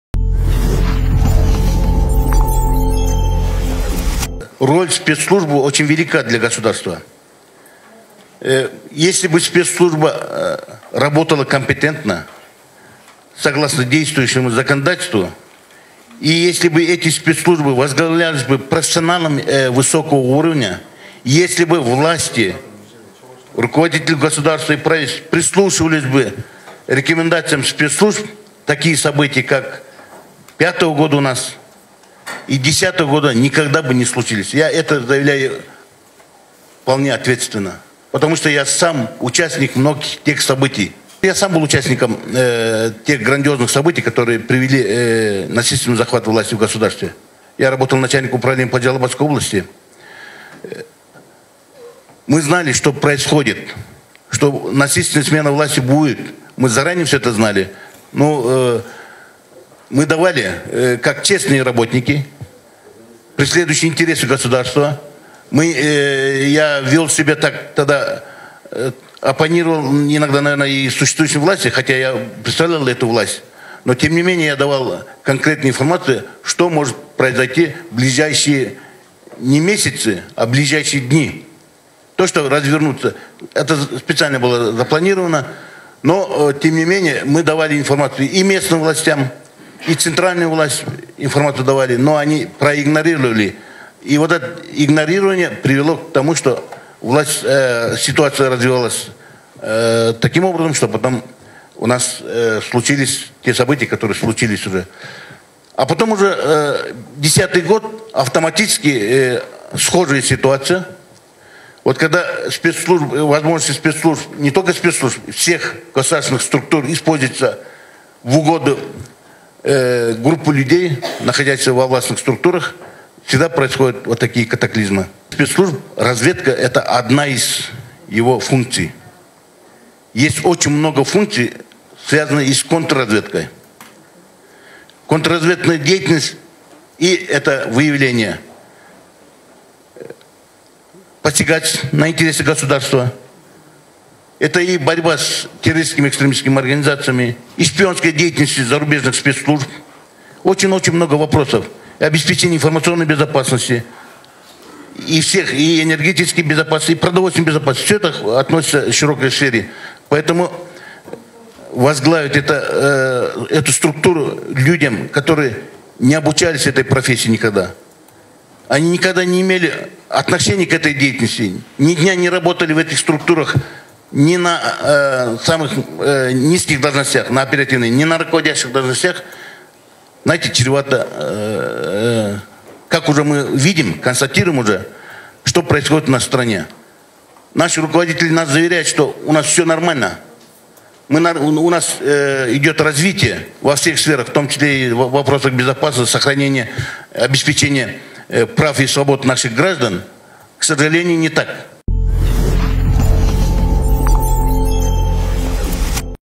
Марат Иманкулов: Доверить человеку кресло спецслужбы
Марат Иманкулов: Доверить человеку кресло спецслужбы, который ни дня не работал в этой сфере чревато последствиями. Мы все свидетели последних событий. В стране тотальные преследования неугодных власти политиков. У ГКНБ должна быть совсем другая работа,которая служит безопастности страны, а не безапсности одного человека.